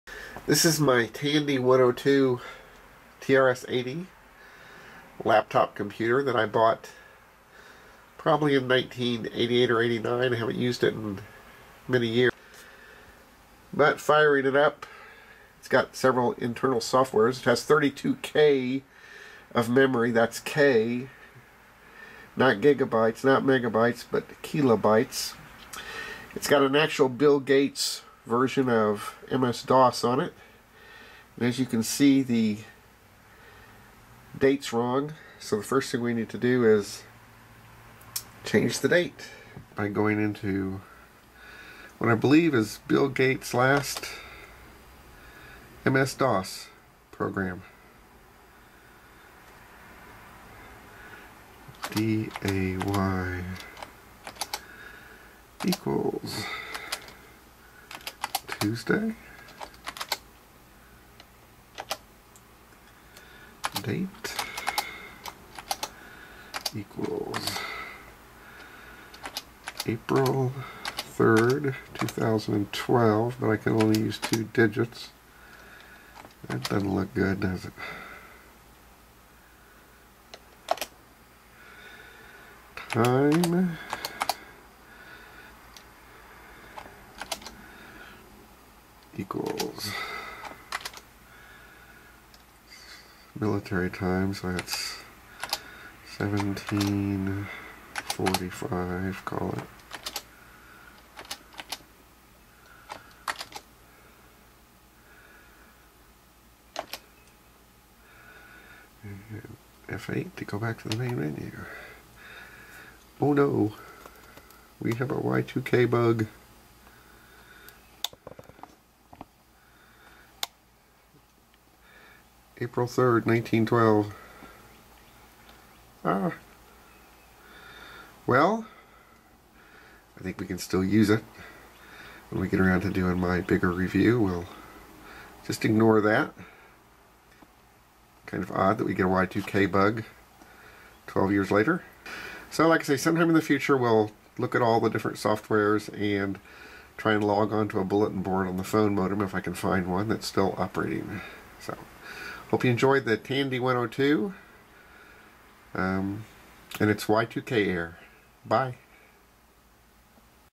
Tandy 102 Portable Computer
I fire up my late 1980's Tandy 102, do a little Basic and find it has a Y2K bug.